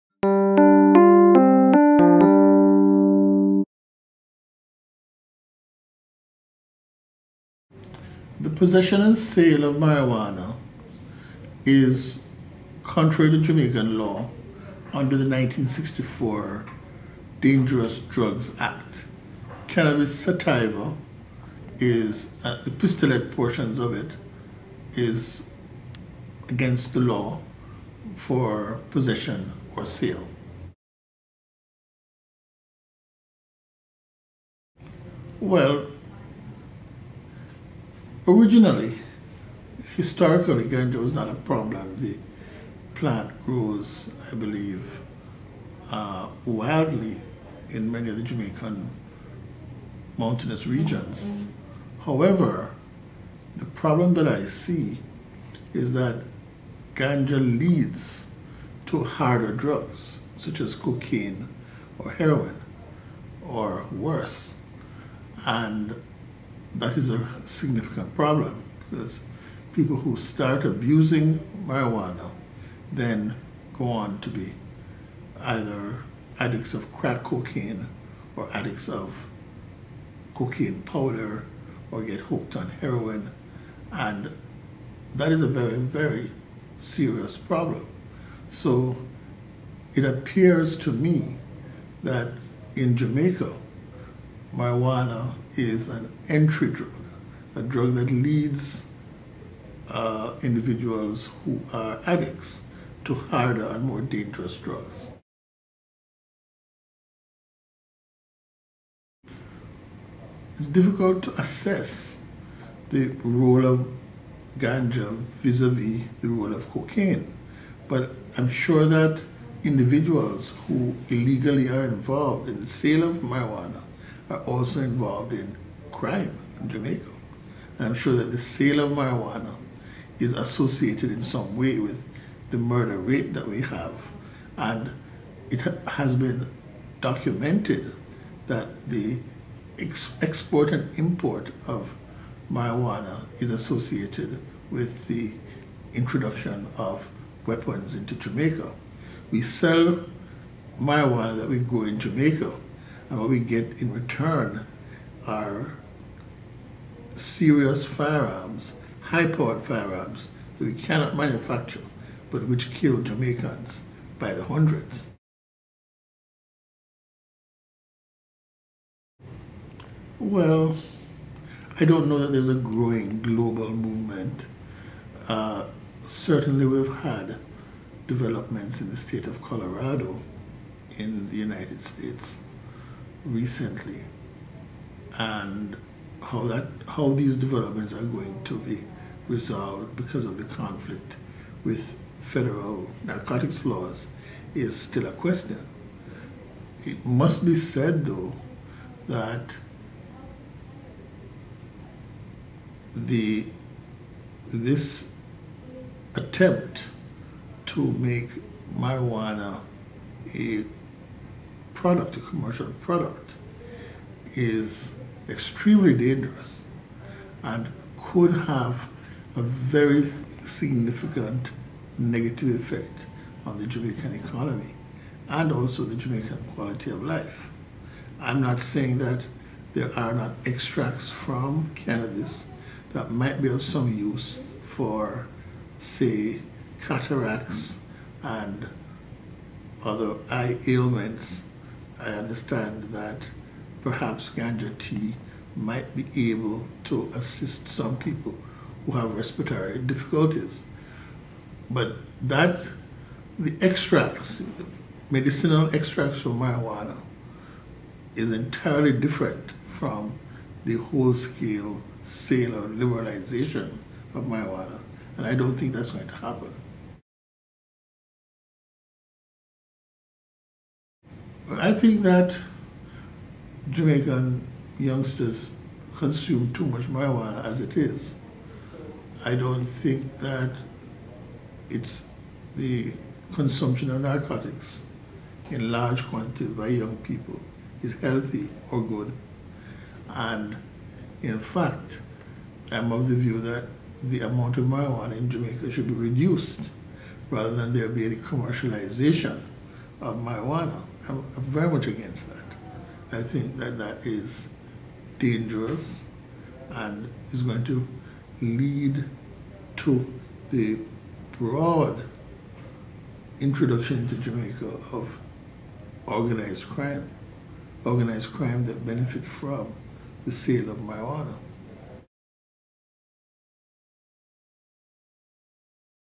David P. Rowe Talks About Marijuana in Jamaica
Professor David P. Rowe speaks about the issue of ganja in Jamaica.